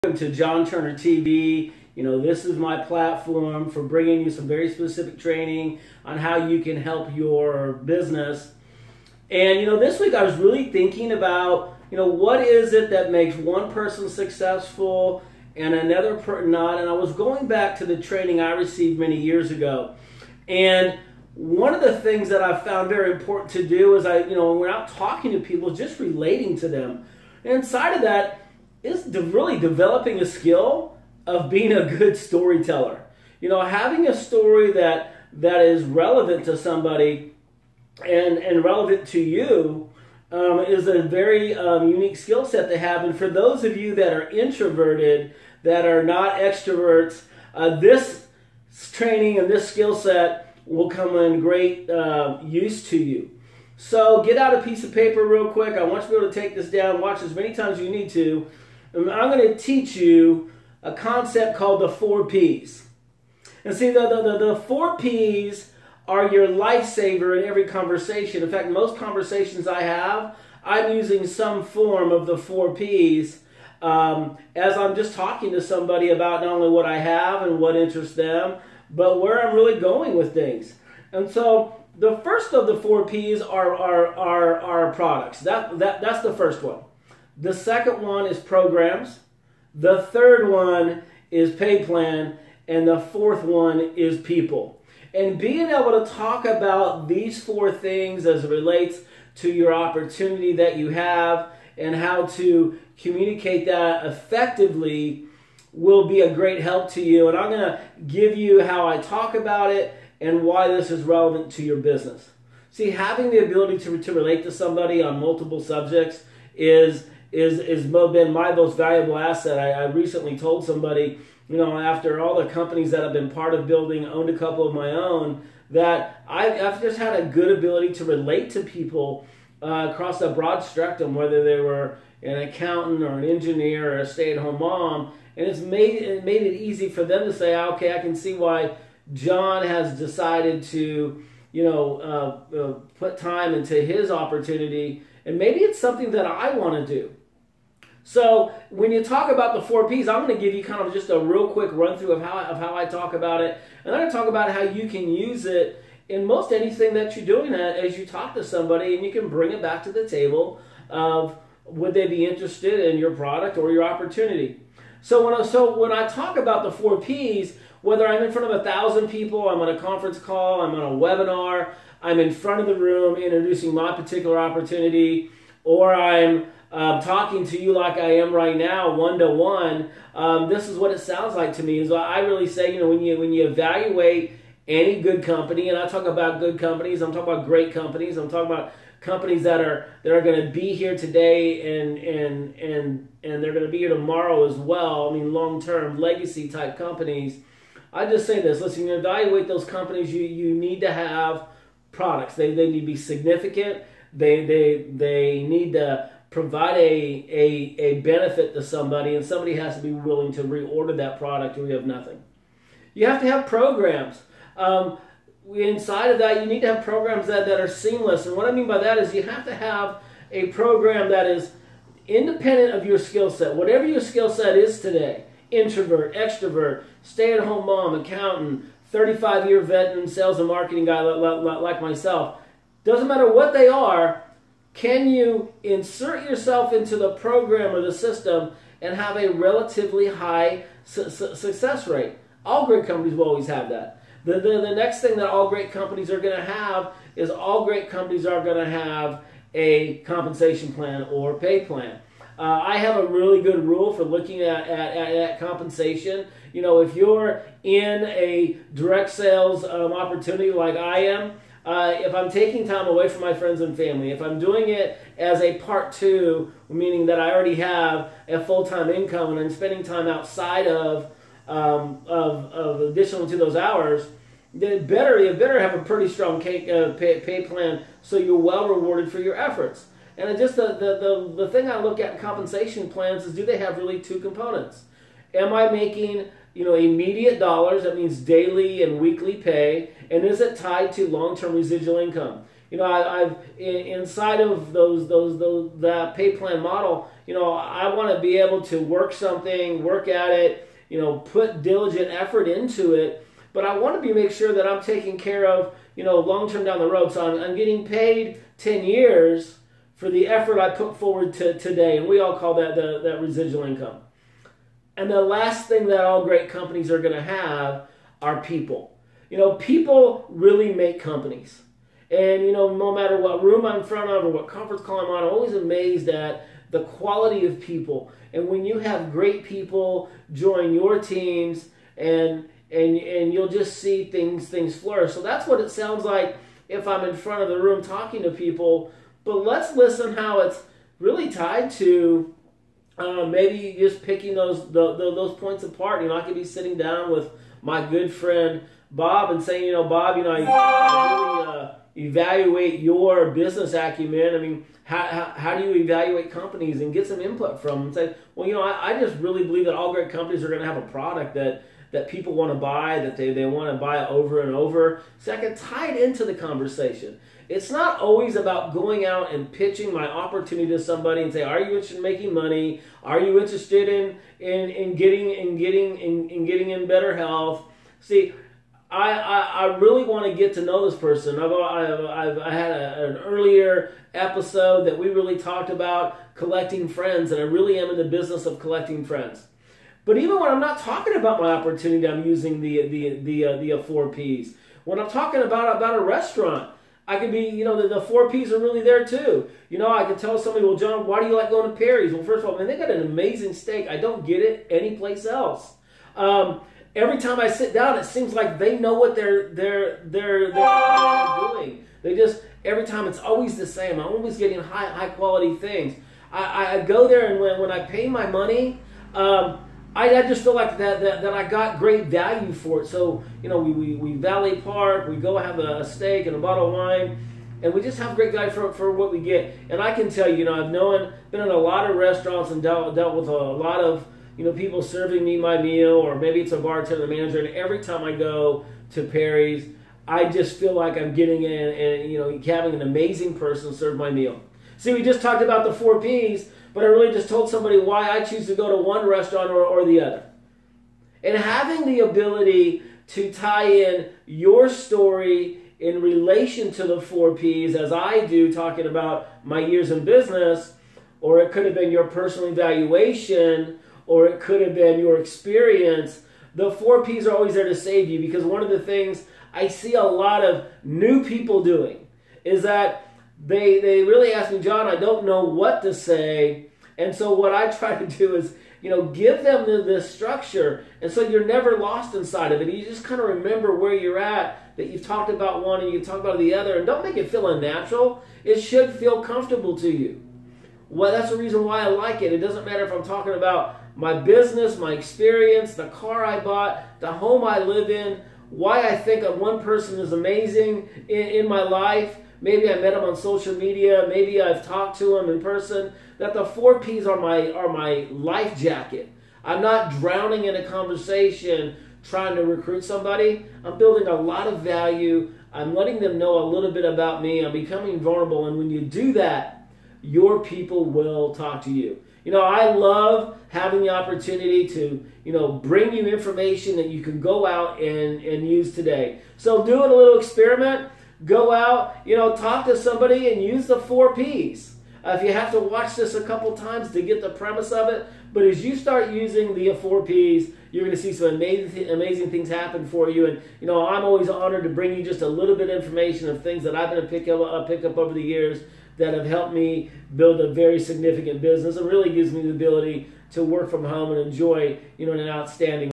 John Turner TV Learn The Four P's
How I use the 4 P's in my business and the value of telling stories.